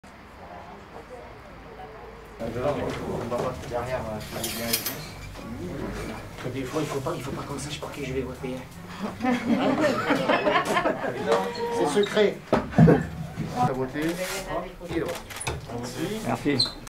Marseille : le candidat RN Stéphane Ravier a voté
Ce dimanche aux alentours de 10 heures, Stéphane Ravier (RN) s'est rendu dans son bureau de vote situé dans le 13ème arrondissement de la ville pour déposer son bulletin dans l'urne.